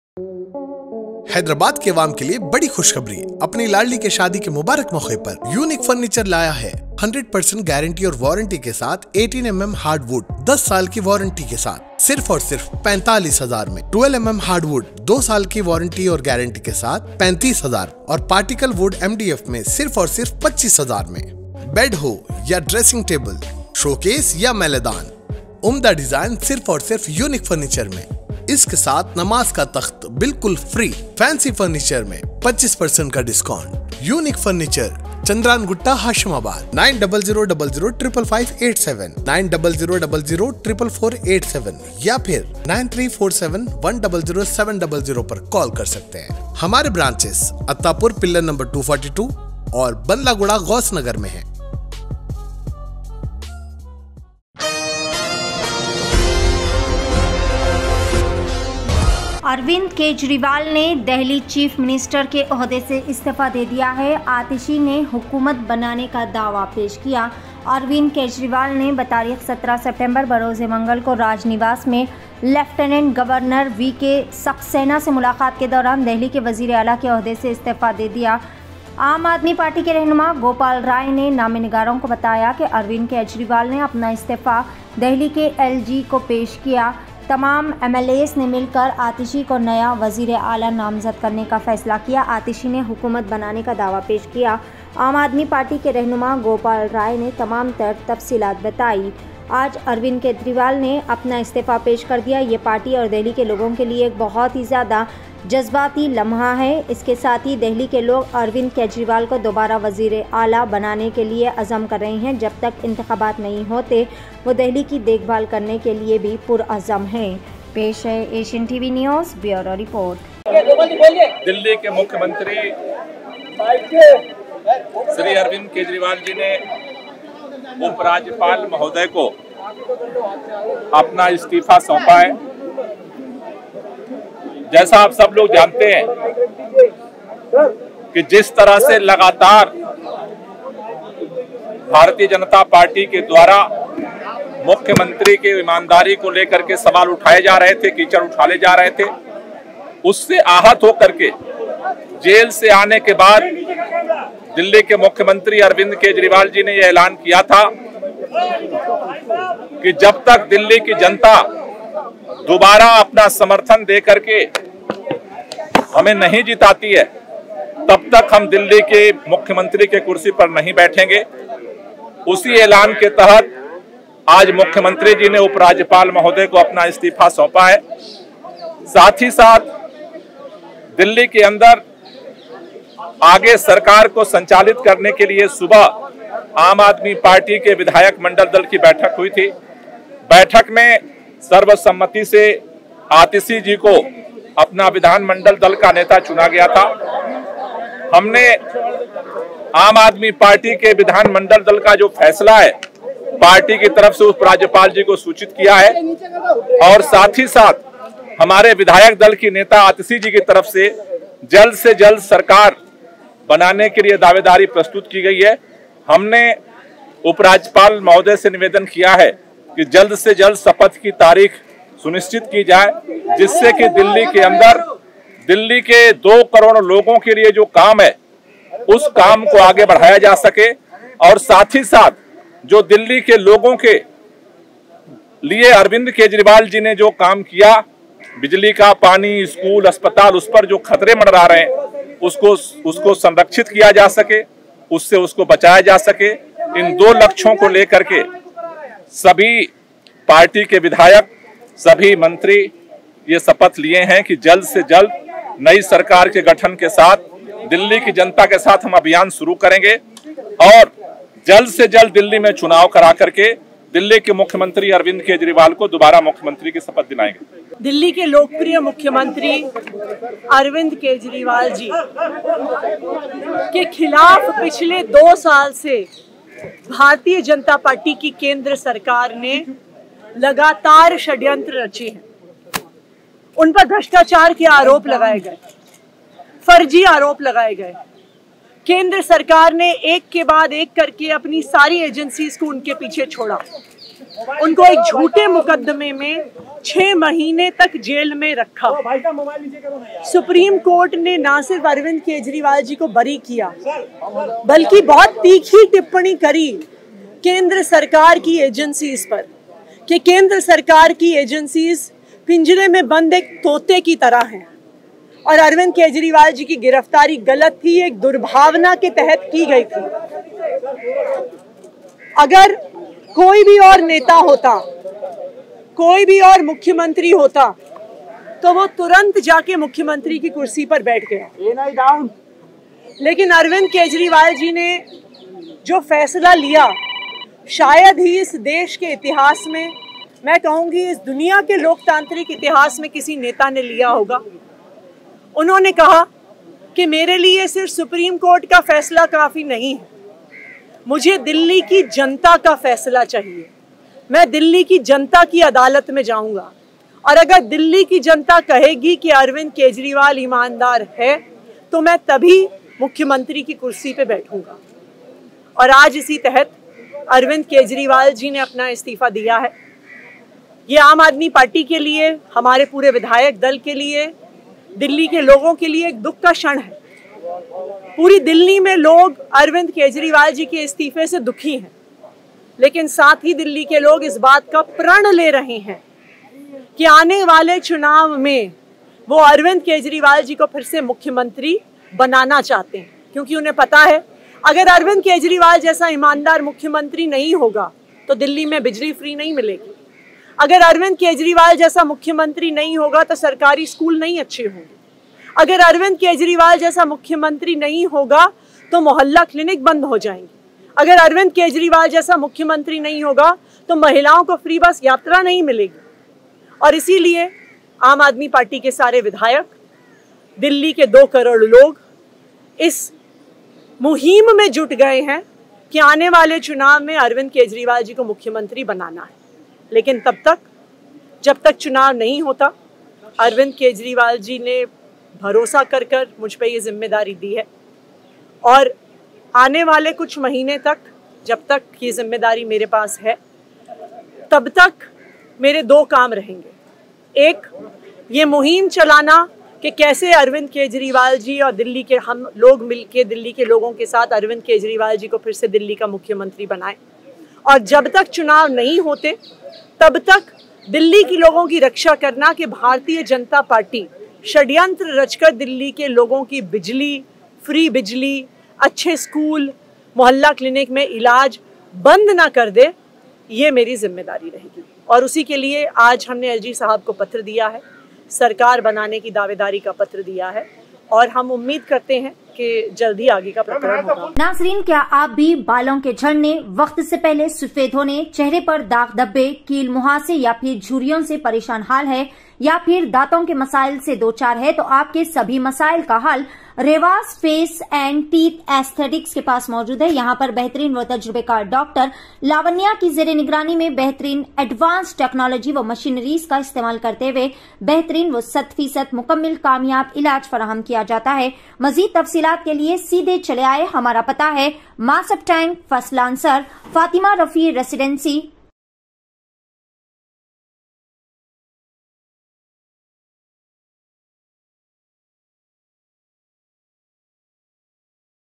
Arvind Kejriwal Resigns as Delhi CM, Atishi Stakes Claim to Form Govt | Asian Tv News
Arvind Kejriwal Resigns as Delhi CM, Atishi Stakes Claim to Form Govt